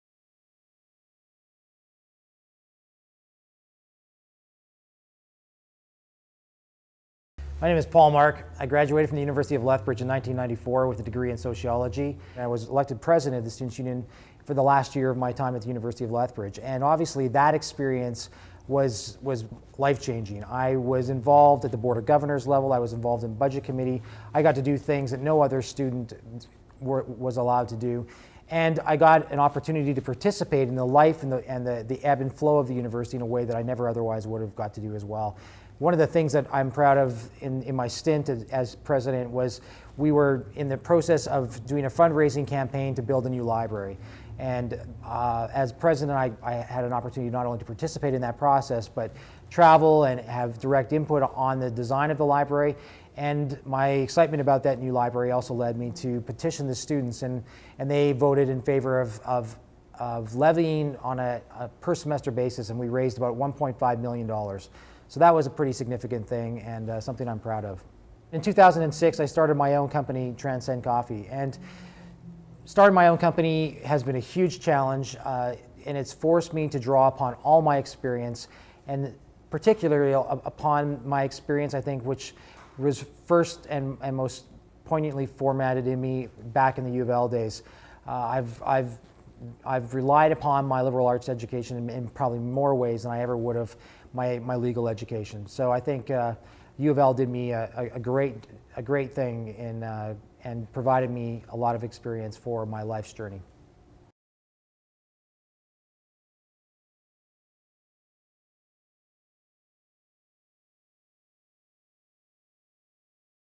Poul Mark - Former ULSU President and Bachelor of Arts Recipient
Poul Mark, who earned a BA in Sociology from the U of L in 1994, now owns and operates Transcend Coffee in Edmonton. He speaks about his experience with the ULSU  and helping shape the face of the U of L Library.